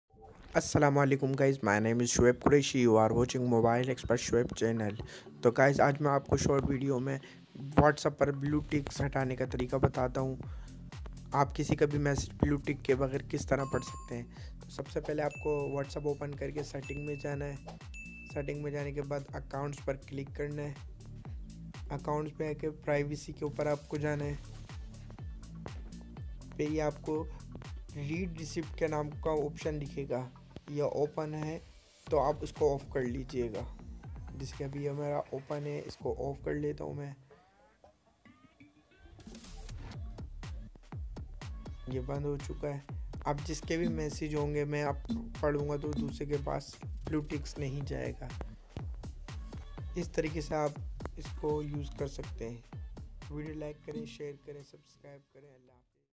Whatsapp blue ticks disable | short video | how to disable blue tick | Mobile Expert shoaib
welcome to a new short vedio
today I will show you . how to disable blue tick on whatsapp

whatsapp blue tick disable
how to disable blue tick
mobile expert shoaib
video